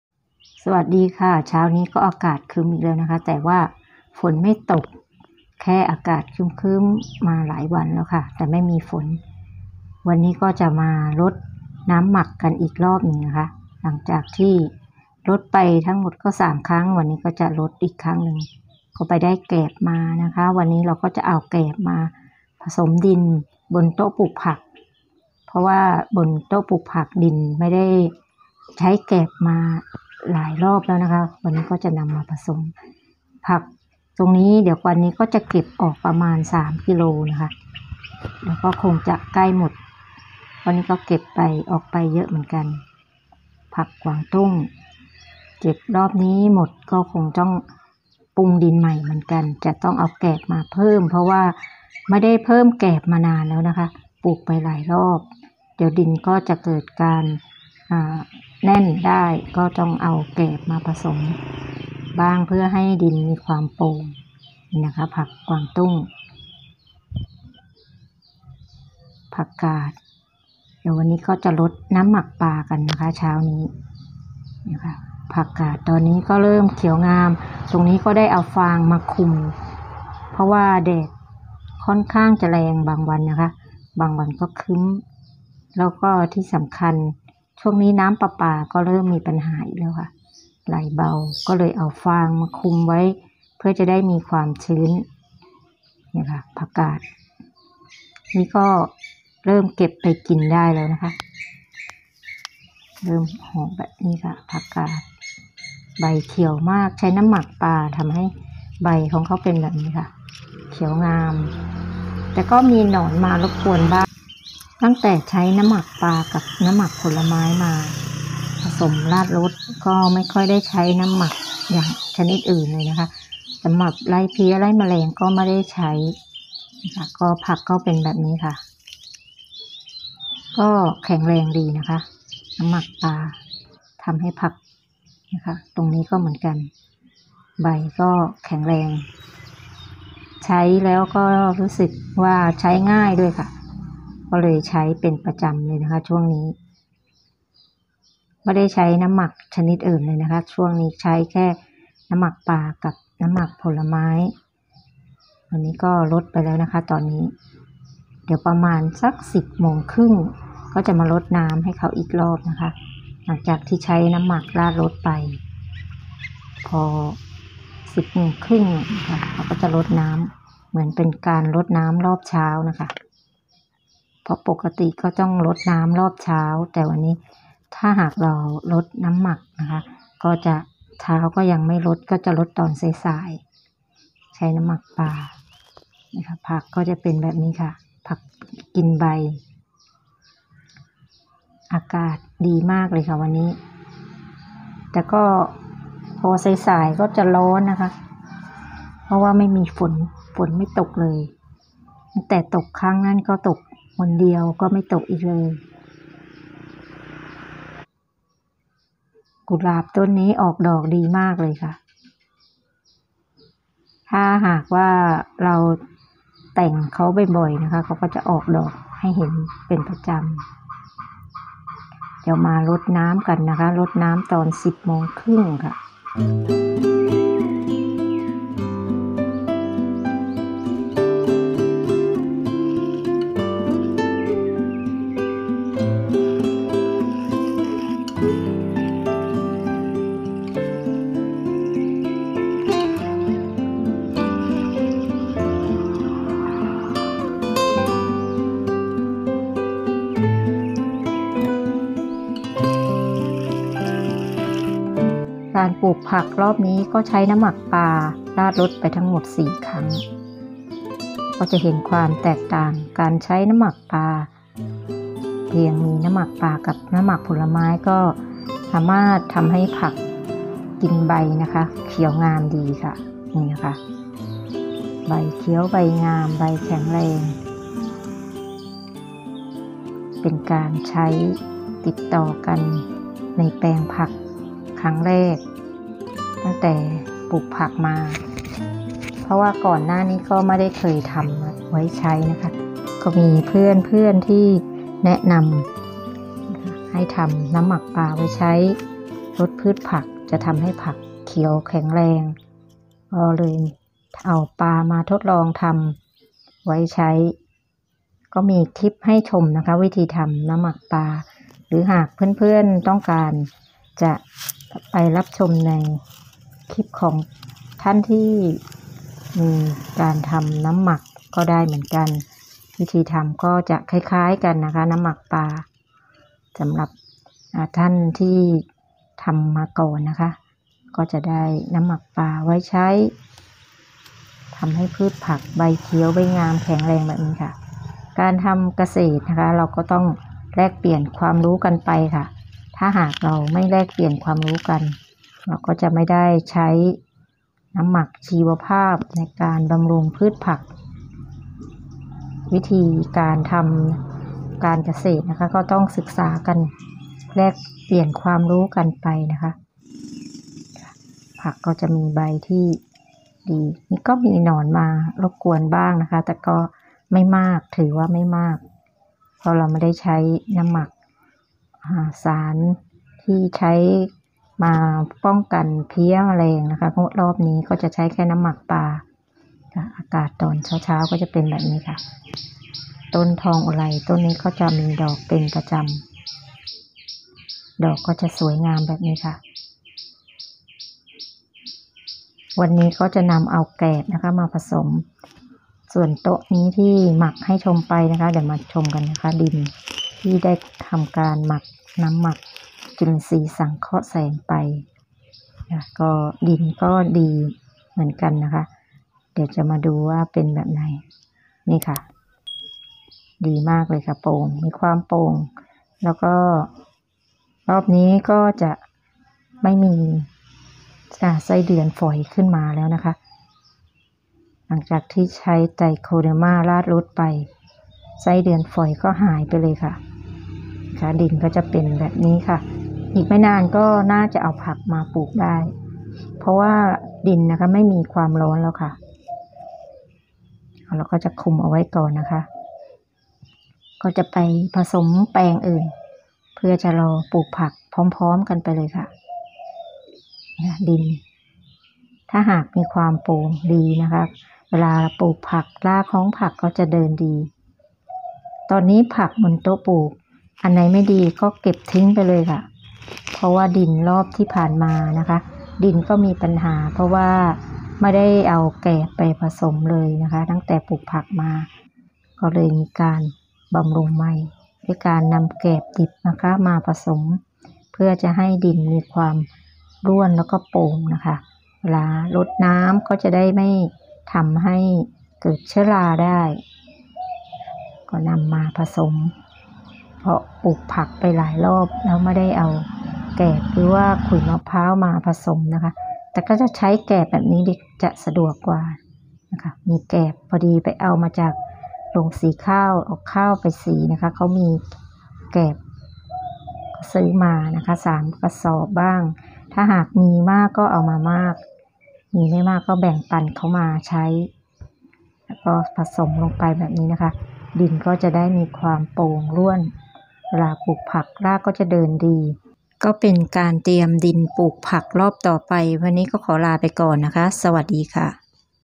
เพิ่มแกลบดิบบนโต๊ะปลูก บำรุงผักอีกรอบก่อนเก็บ @วัลย์ สวนครัว
วิธีเพิ่มแกลบดิบบนโต๊ะปลูกช่วยทำให้ดินร่วนรากผักจะได้เดินดีขอขอบคุณทุกกำลังใจที่ติดตามรับชมช่องวัลย์ สวนครัวขอบคุณมากค่ะ